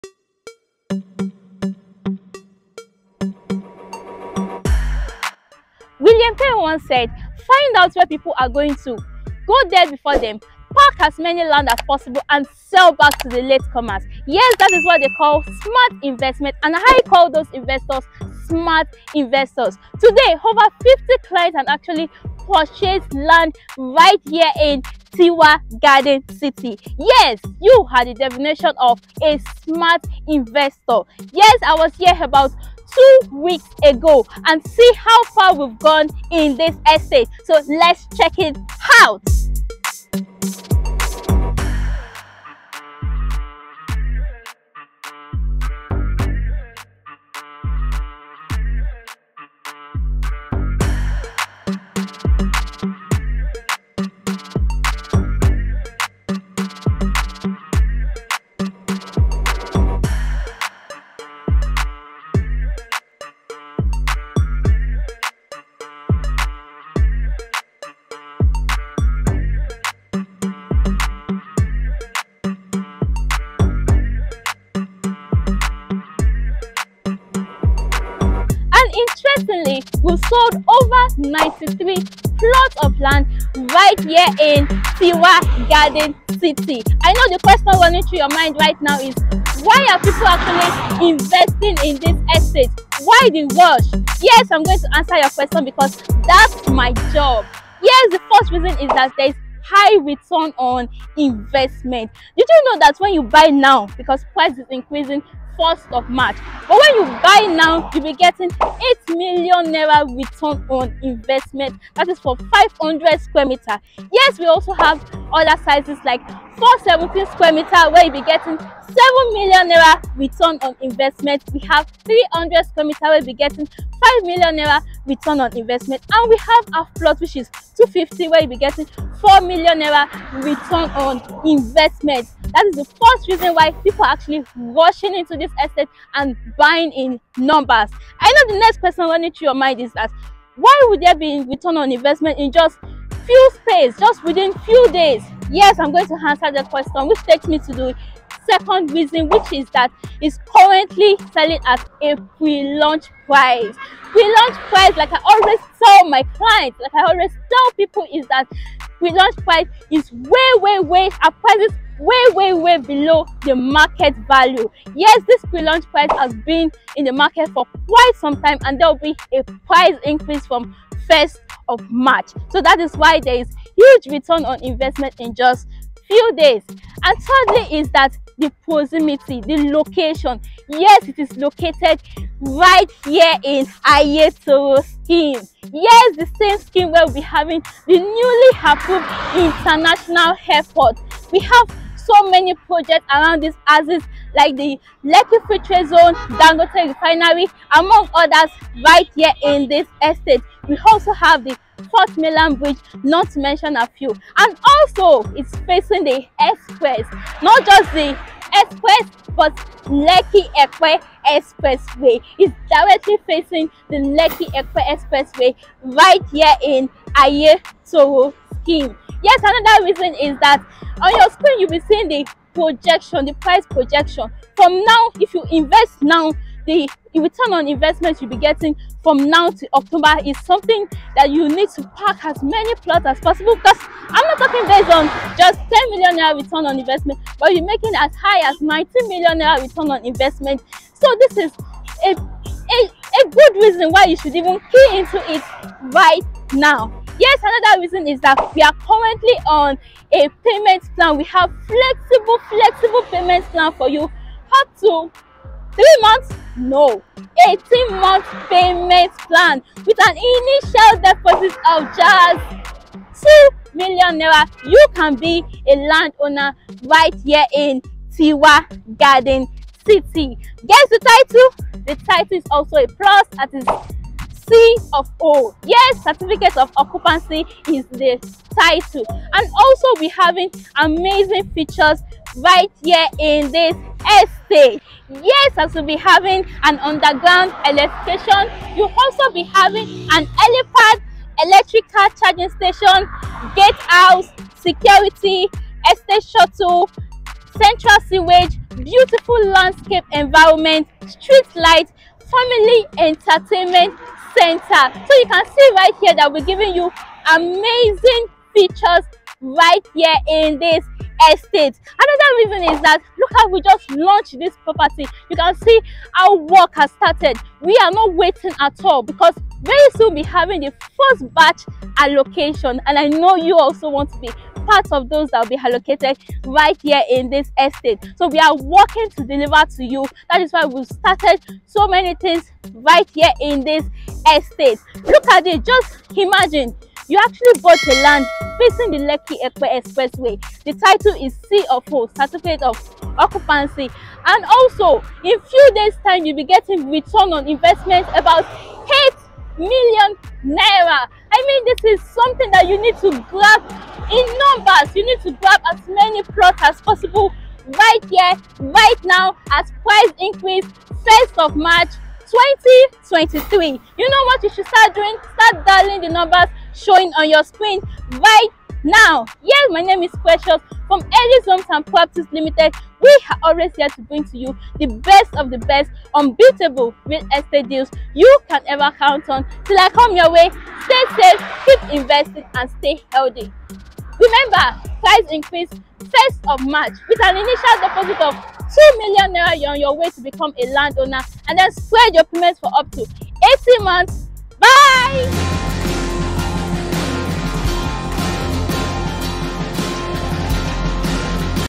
Land in Ibeju Lekki Lagos || Land for Sale in Lagos
OPPORTUNITY TO LOCK THE FUTURE TODAY

INTRODUCING TIWA GARDEN CITY ESTATE

Have you ever thought about living in an estate where you can have it all?

To have and enjoy the 3 way unique experience with Family- Work-Life .

You sure can have all of that and more in the Smart City for Smart People; Tiwa Garden City

Tiwa Garden estate not only guarantees you the work-life balance you deserve but also brings to you a perfect blend with technology and nature

Here is where quality living lies as you achieve quality work and raise quality family while spending quality time in the first class facilities that the estate has for you .

Located in the heart of the new Lagos in Ibeju Lekki, Tiwa Garden City secures you closure to everywhere you want to be from the bubbling areas in Ibeju-Lekki Community to the Ancient town of Epe, the academic hubs like Pan Atlantic University and Lagos Business School to the easy access to Lekki International Airport for your Travels and the Lekki Free Trade Zone, Sea Port, Refinery for business opportunities

LOCATION: Aiyetoro, Few Minutes to new international airport, along Lekki-Epe Expressway, Lagos

Secure your own box of luxury today at TIWA GARDEN CITY by choosing any of the following options at the Pre-Launch price👇👇👇

✅250 SQM  selling at N9.5million
✅300SQM selling at N12million
✅417SQM selling at N16million
✅500SQM selling at N19million

Initial deposit is 2million naira and you can spread payment for up to 18months.

This is your chance to lock in the future, buy Tiwa Garden City today‼️

Halleys Homes and Properties Limited